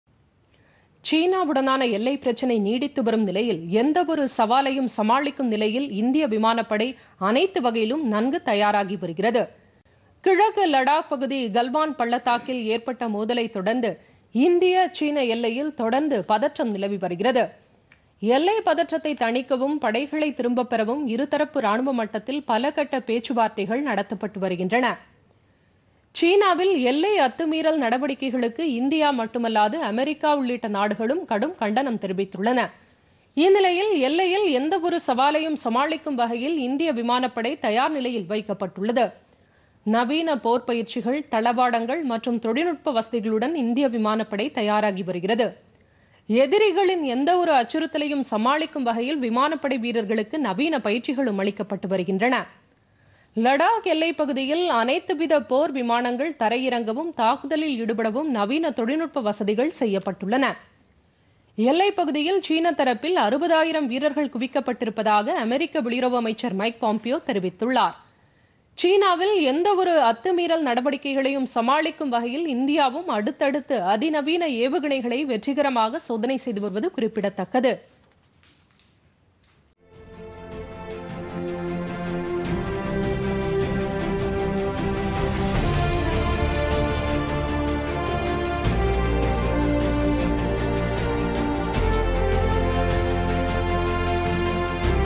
எல்லையில் தயார் நிலையில் இந்திய விமானப்படை
லடாக் எல்லையில் தயார் நிலையில்  இந்திய விமானப்படை   -  எந்த ஒரு சவாலையும் சமாளிக்கும் வகையில் போர் விமானங்கள் தீவிர பயிற்சி.